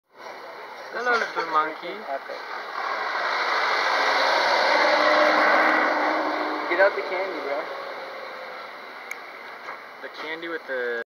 "Hello little monkey"
2014. Misahualli, Ecuador. Just a little monkey looking for something to eat in this little town. Disappointed with us, he went into a restaurant to try his luck there.